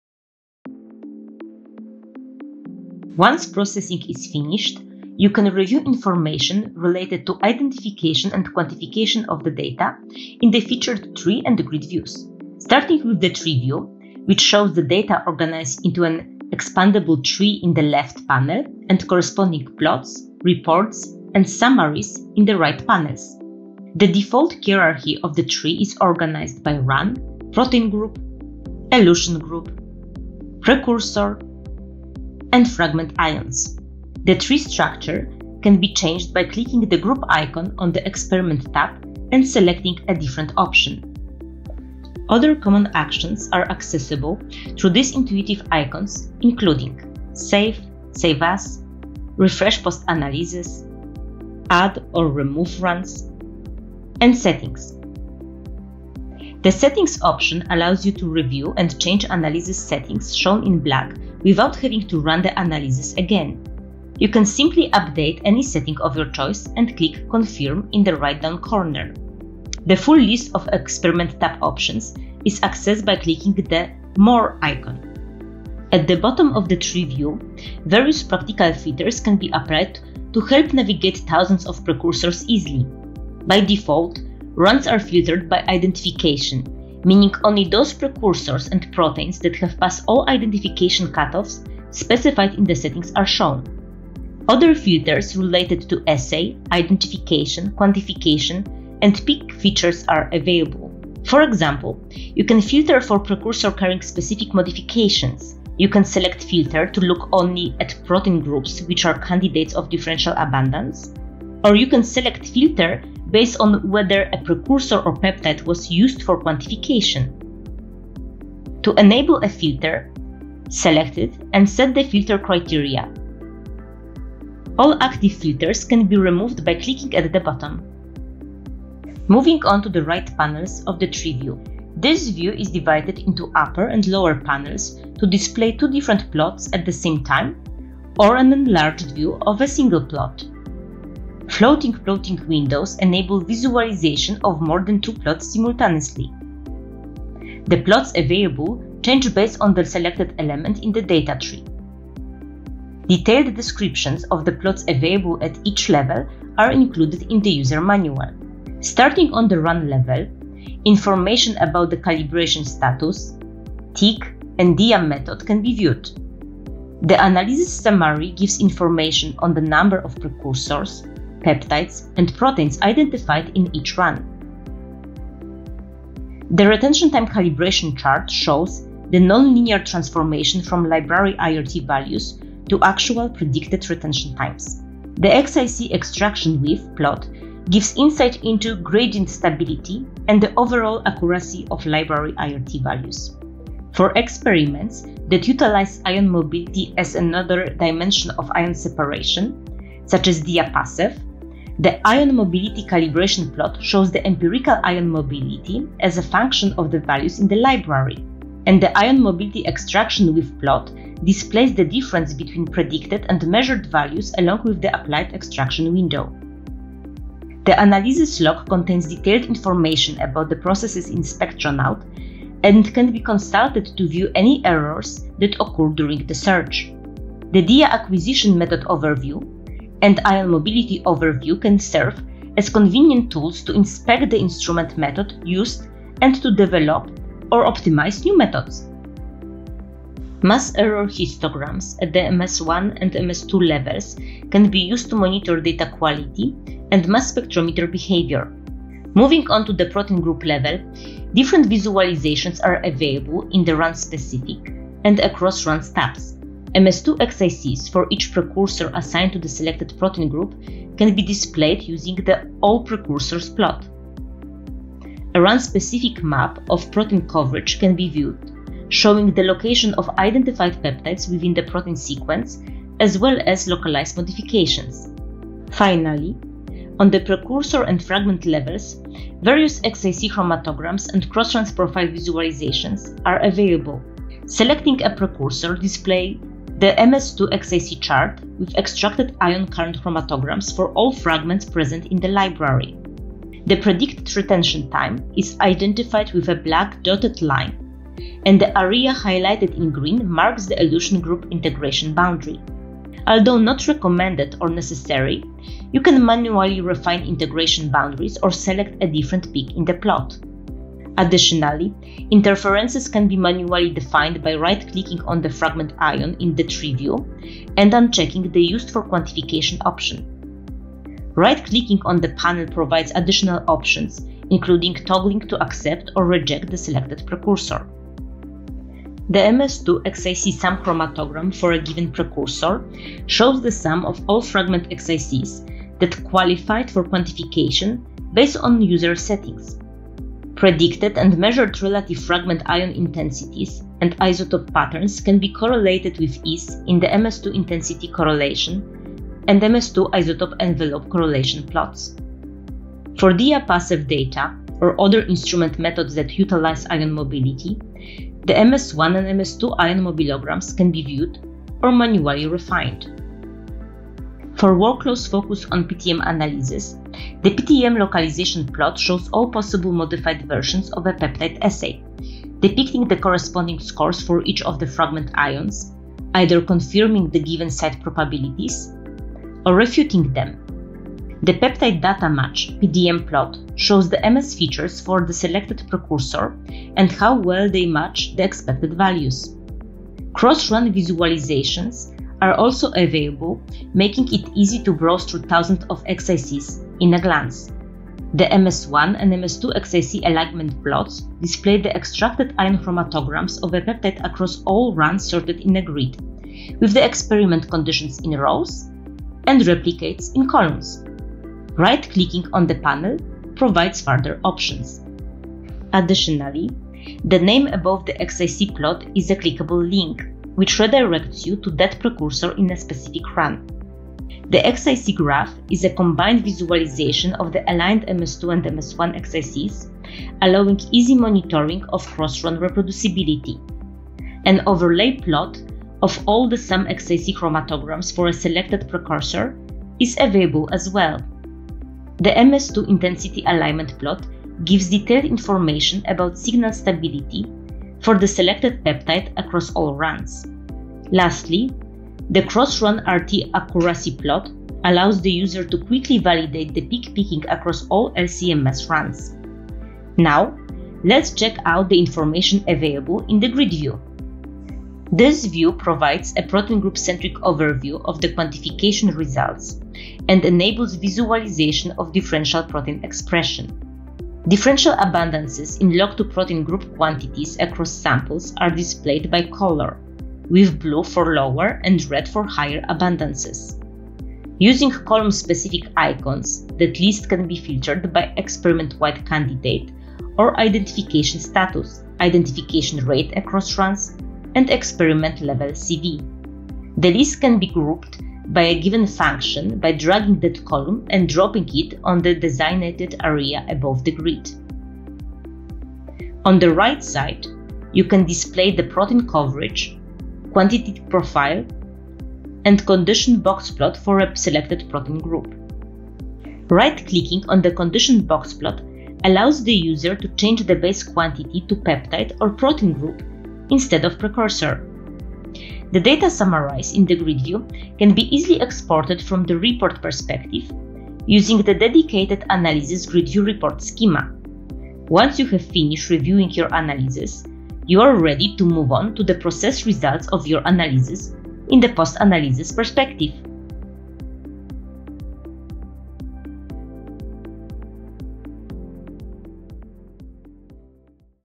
Spectronaut: Analysis Perspective - Reviewing Visualized Data
Introducing our new Spectronaut video tutorial series!👩‍🚀📽️

In these tutorials, Monika Puchalska, our Software Product Manager, walks you through everything Spectronaut has to offer – from software installation to advanced data visualization and analysis.

Whether you're a seasoned user or just starting, our tutorials cater to all levels, ensuring you can unlock the full potential of Spectronaut.